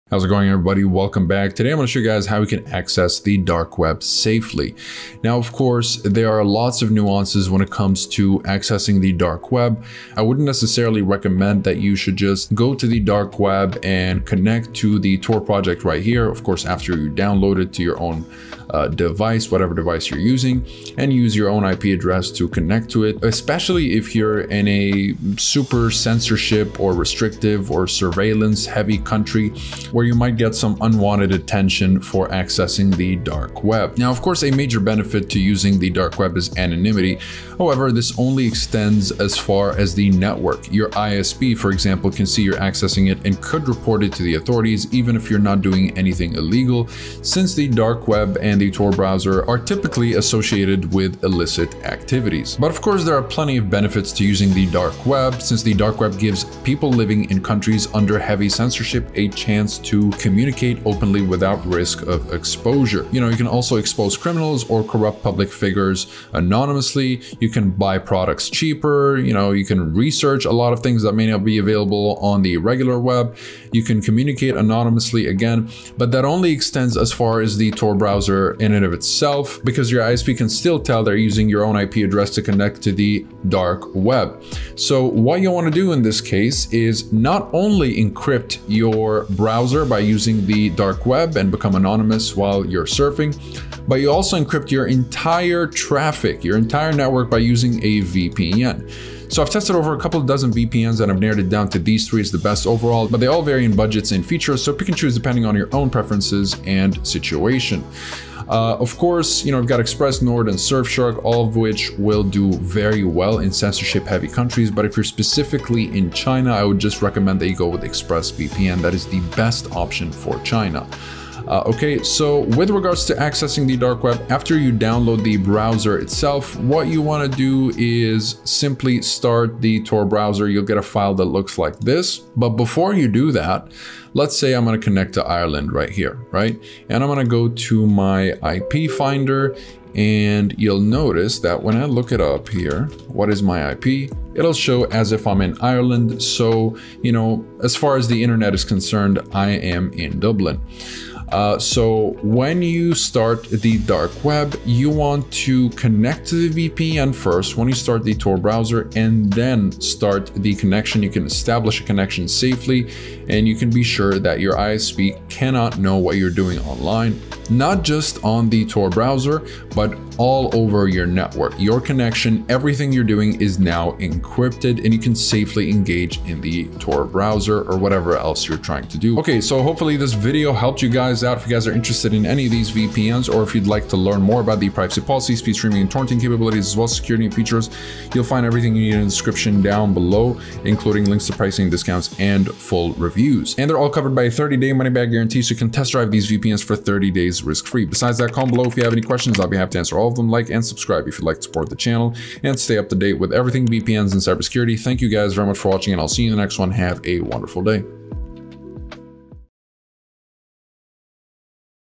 How to Access the Dark Web Safely in 2026: An Easy Guide
This video provides a guide on accessing the dark web safely, emphasizing the importance of caution, especially in censorship-heavy or surveillance-prone countries where dark web activity might attract unwanted attention. While anonymity is a key benefit of the dark web, it's essential to understand the potential risks, including ISP monitoring and association with illicit activities.

To bolster security and privacy when accessing the dark web, the video recommends using a VPN in conjunction with the Tor browser. Three VPN options are highlighted: ExpressVPN, NordVPN, and Surfshark, each offering varying features and suitable for different budgets and needs.

Viewers are encouraged to explore the VPN options mentioned for enhanced security when accessing the dark web or engaging in other online activities.

Hope you enjoyed my How to Access the Dark Web Safely in 2026 : An Easy Guide Video.